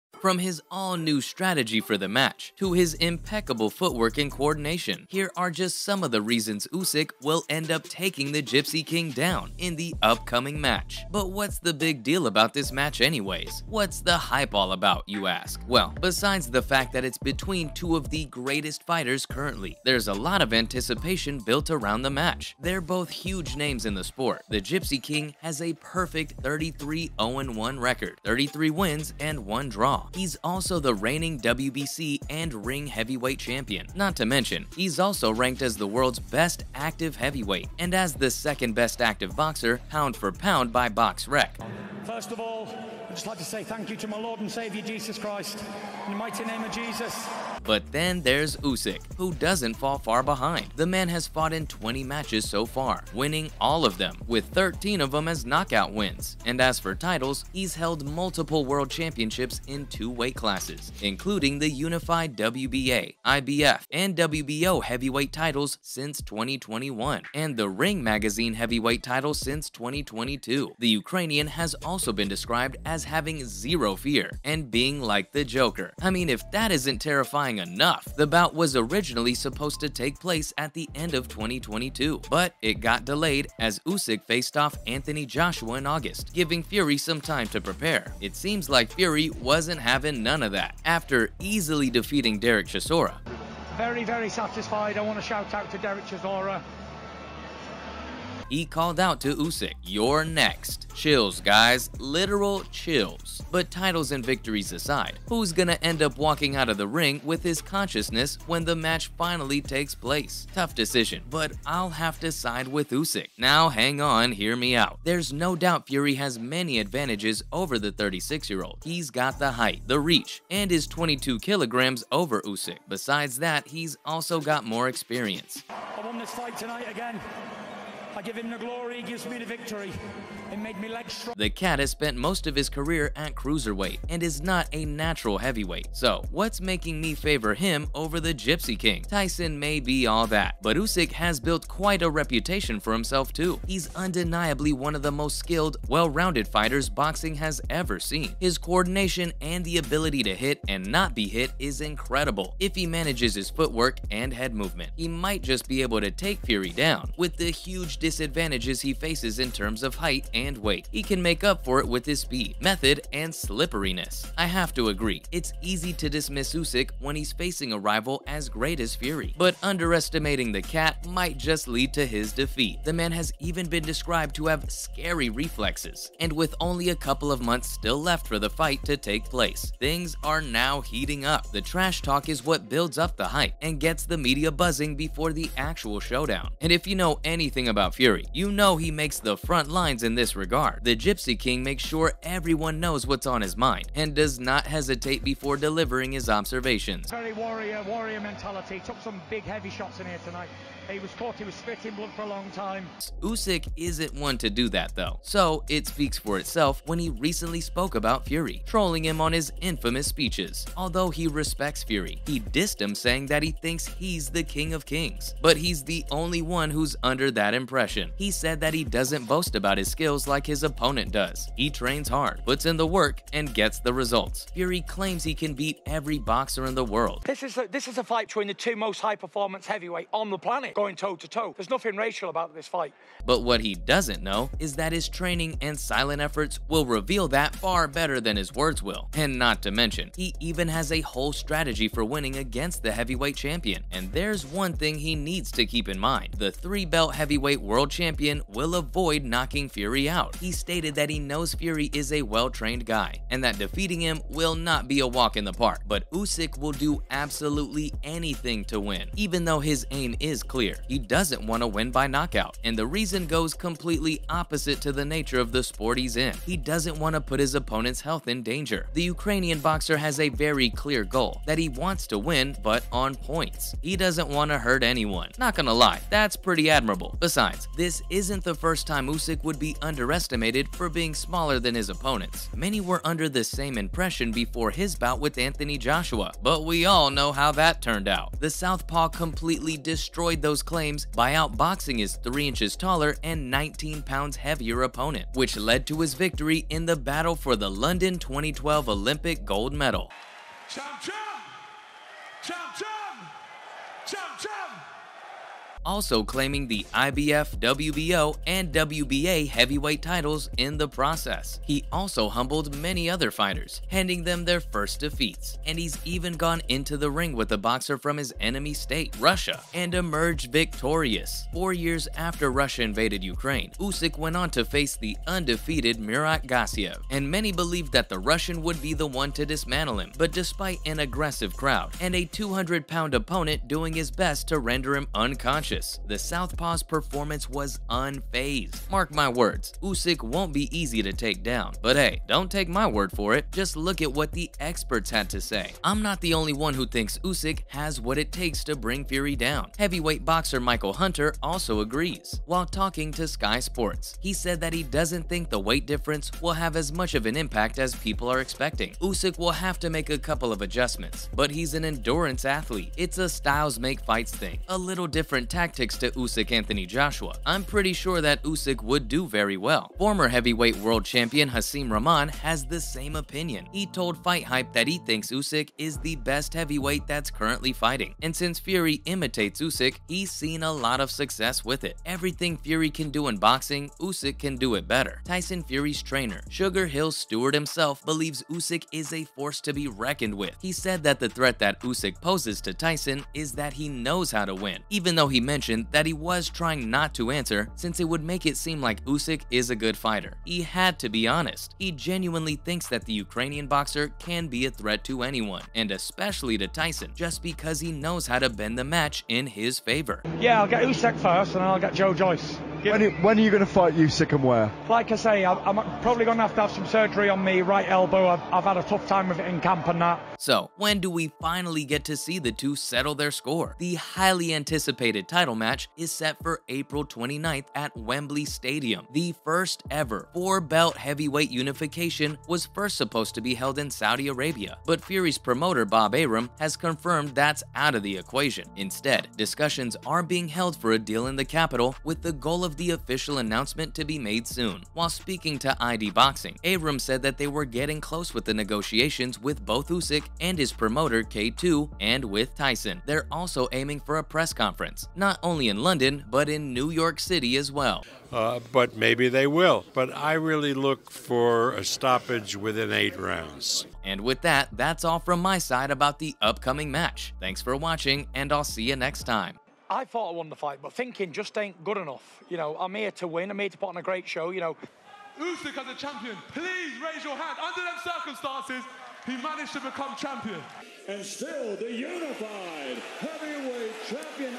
Oleksandr Usyk Will DOMINATE Tyson Fury.. Here's Why
Welcome back to Sports Spam, today on the channel we're going to be talking about Oleksandr Usyk Will DOMINATE Tyson Fury.. Here's Why if you're excited to learn more about this then make sure you watch the whole way through because you don't want to miss the details we have for you!